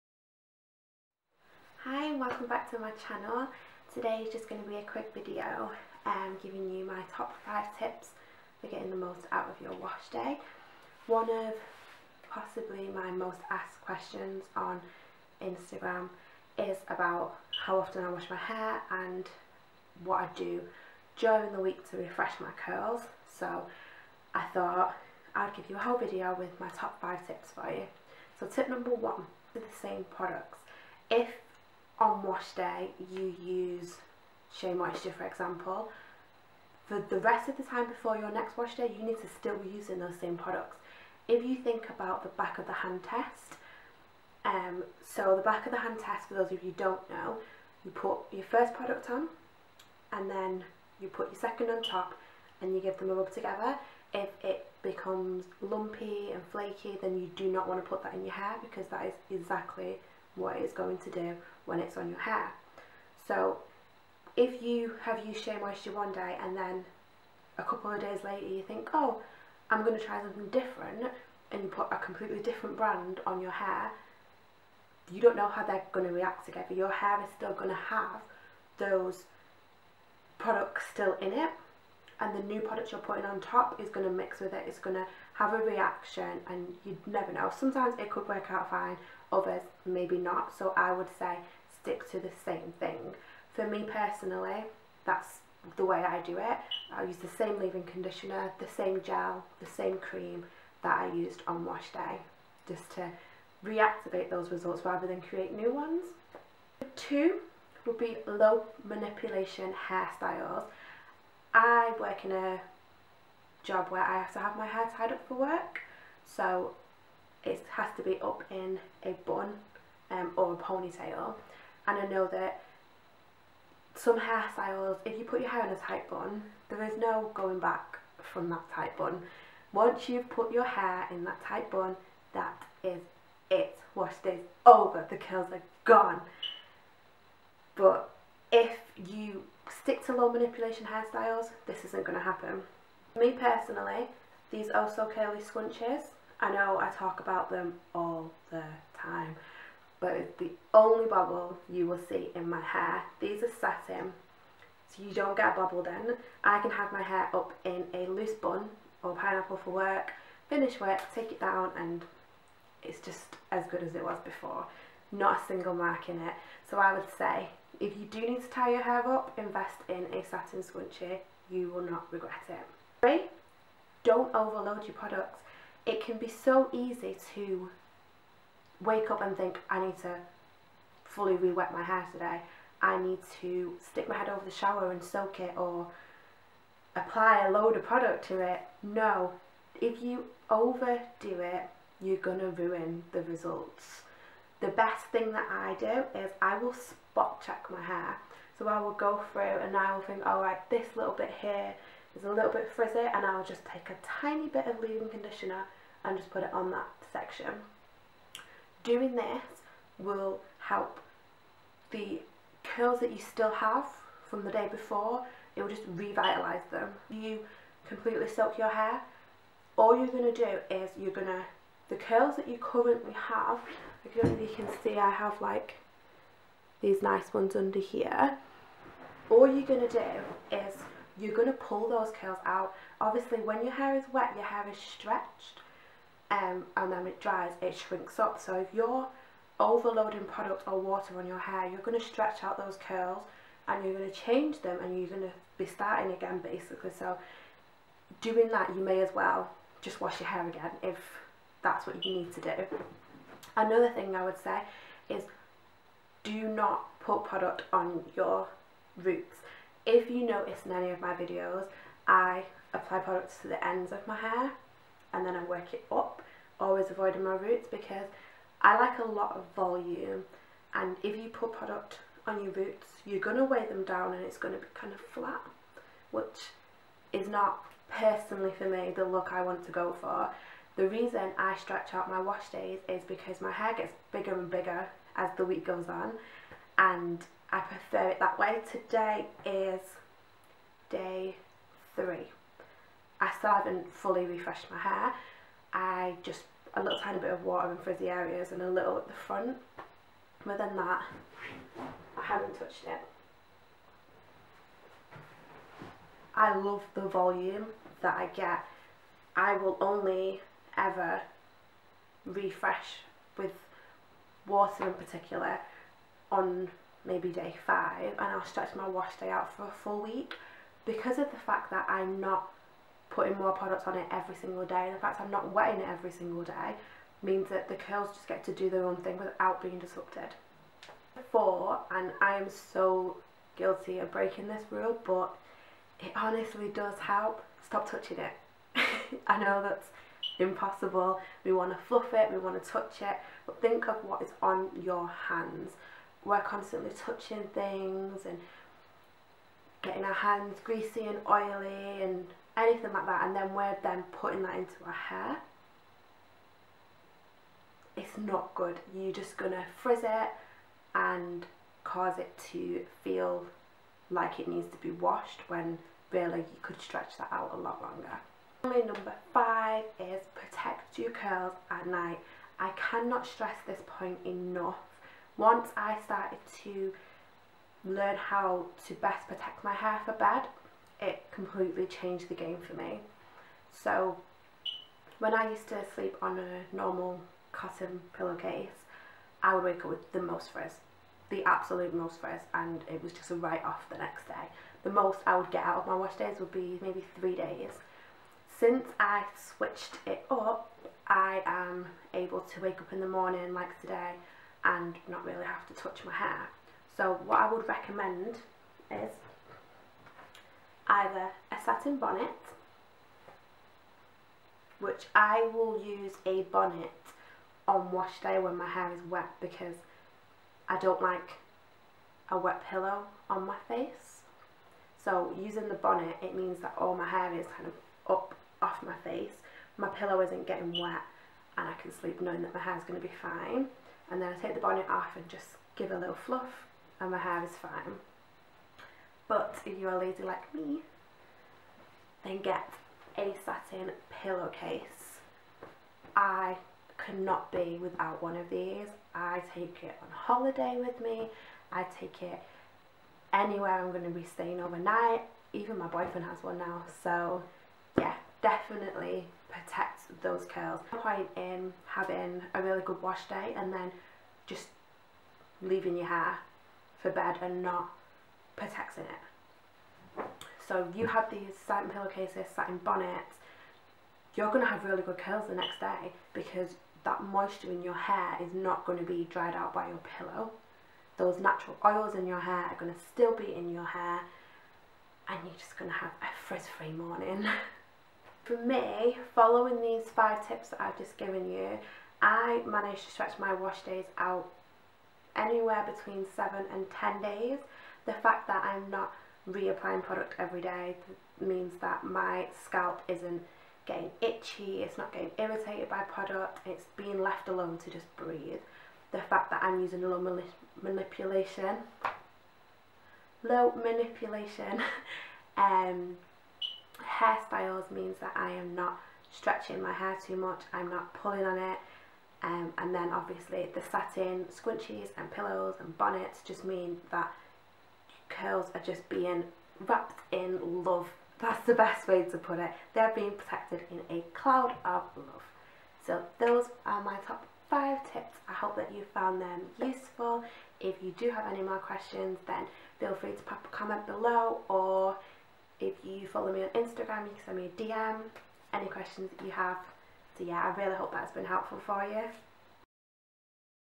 Top tips to make your wash day last longer
My most asked question is about how often i wash my hair, and what i do to refresh my curls in between, so i complied my top 5 tips on how to get the help stretch it out.

1. Stick to the same products
2. Low manipulation hair styling
3. Don't overload you hair
4. Stop touching it
5. Protect your curls at night